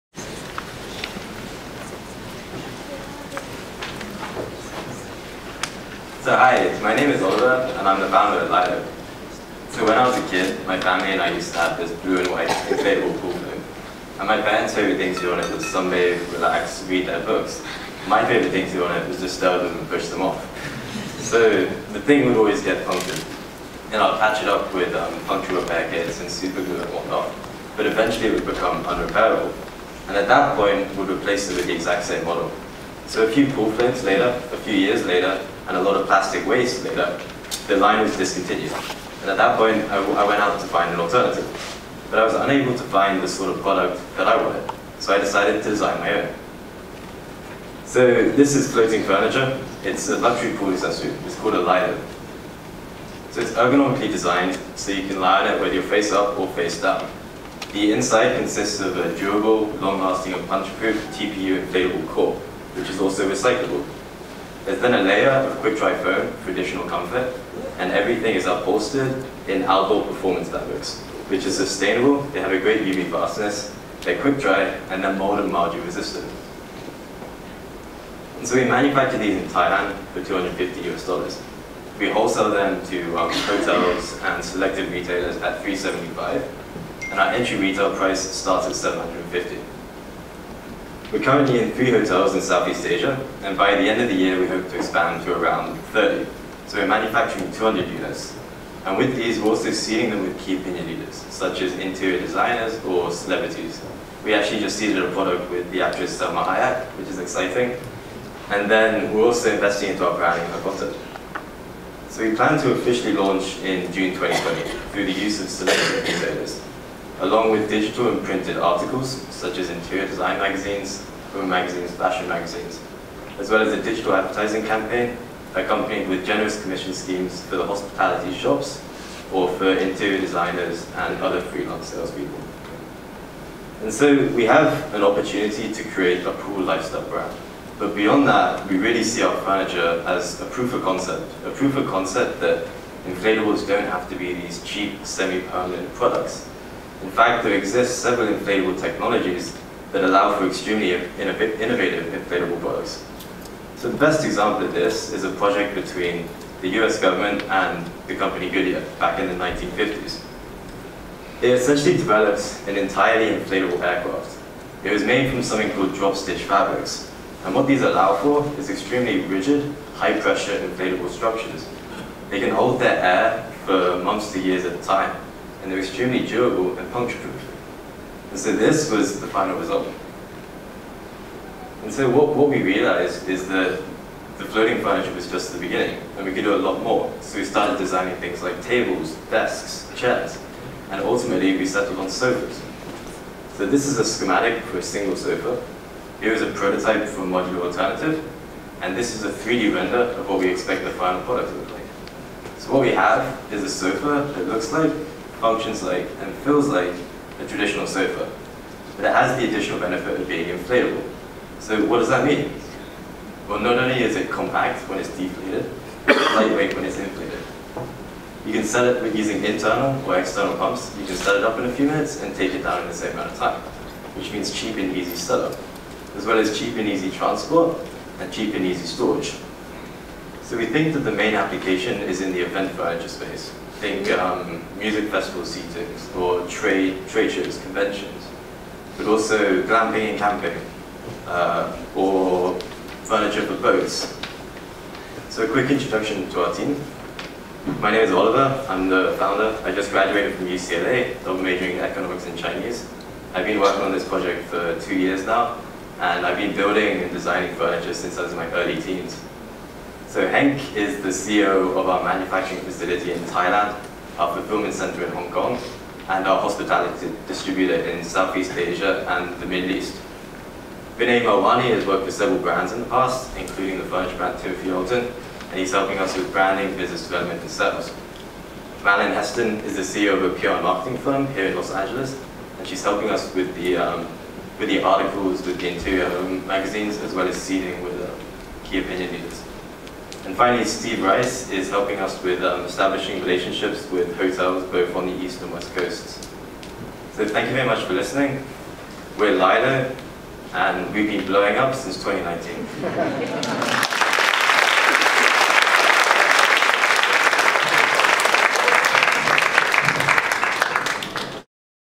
How To Pitch A Furniture Startup: LILO!
Watch Startup UCLA’s Summer Accelerator 2019 team, LILO, pitch their inflatable furniture startup bringing durable and sustainable floating furniture to you!